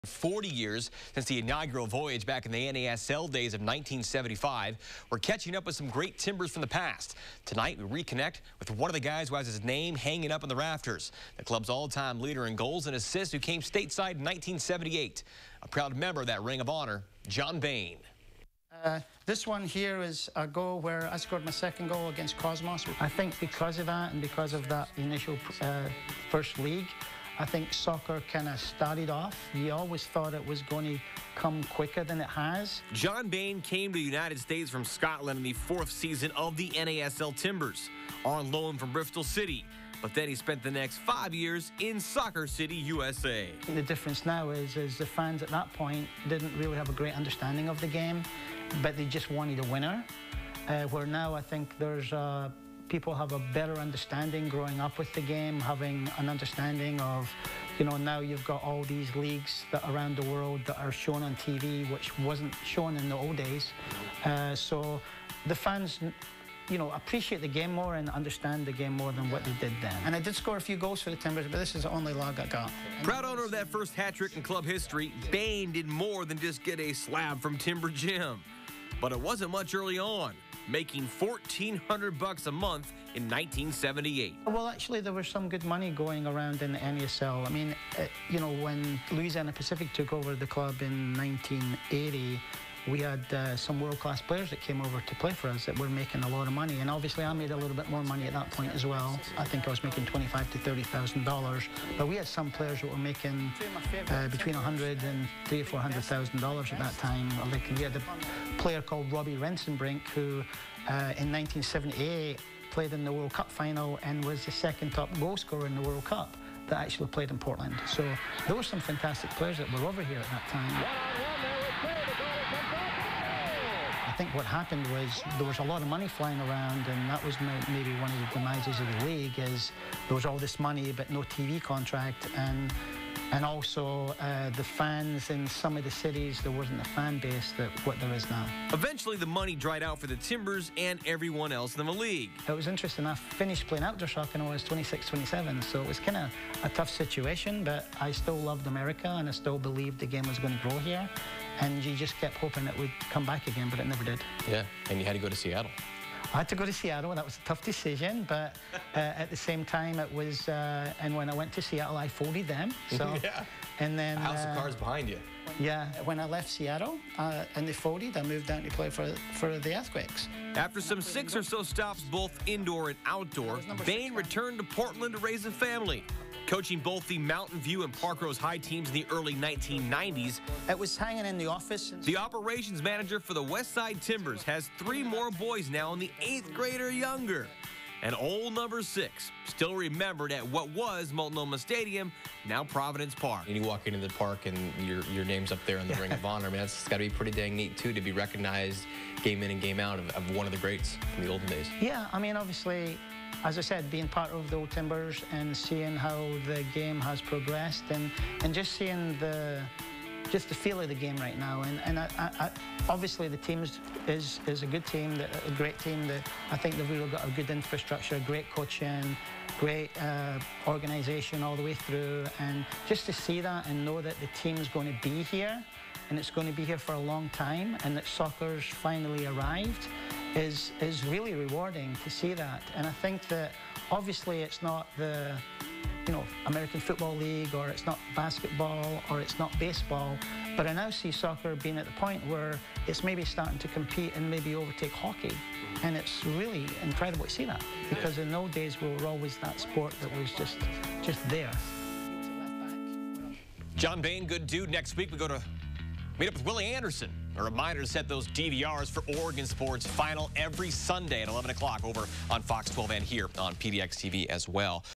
Timbers at 40: John Bain
Timbers Ring of Honor member John Bain talks to Fox 12's Nick Krupke about his first year in Portland, 1978 with the NASL club.